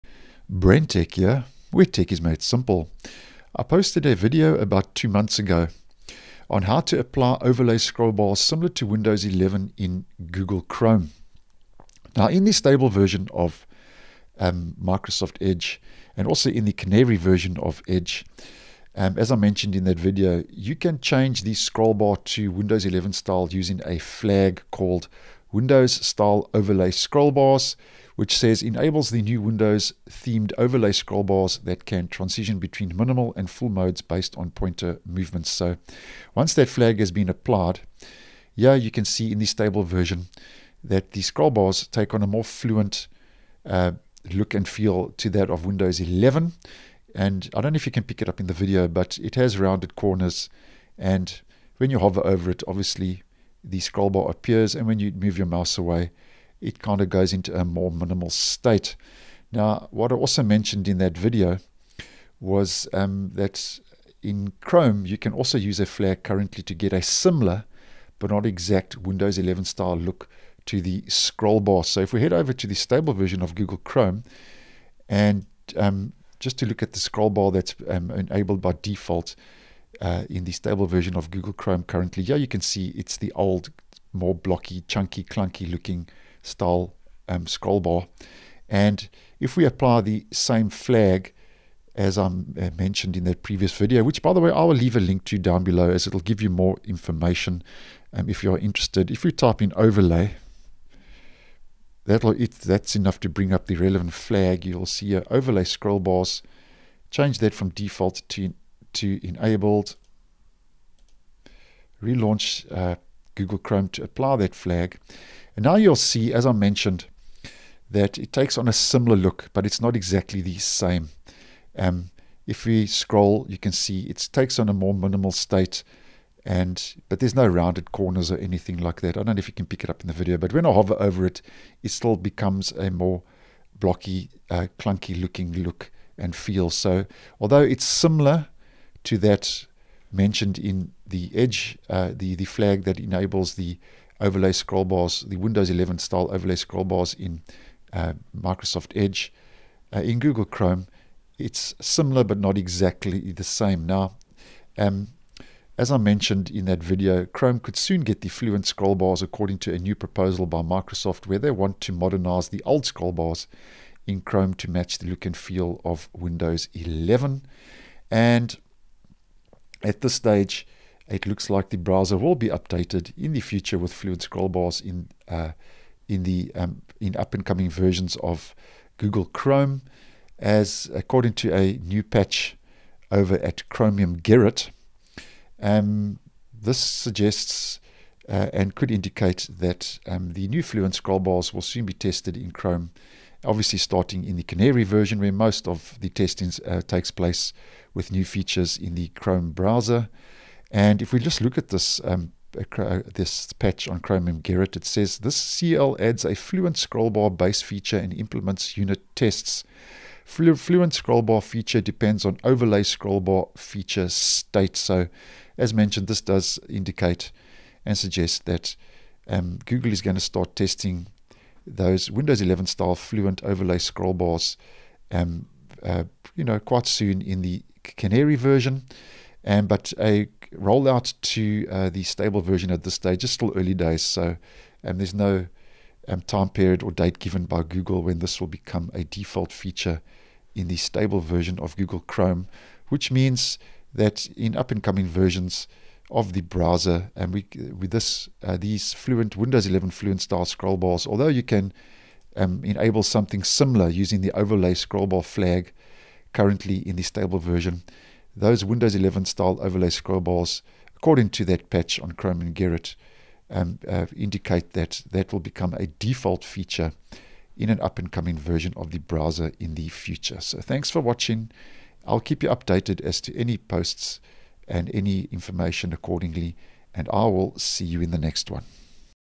Google Chrome could be getting Windows 11 style "Overlay scrollbars" shortly
In Microsoft Edge you can change the scrollbar to Windows 11 style using a flag, while in Chrome you can also use a flag currently to get a similar , but not exact Windows 11 style look to the scrollbar. Chrome could soon get the Fluent scrollbars, according to a new proposal by Microsoft, where they want to modernize the old scrollbars in Chrome to match the look and feel of Windows 11. Now it looks like the browser will be updated with the Fluent scrollbars in the near future according to a new patch in Chromium Gerrit.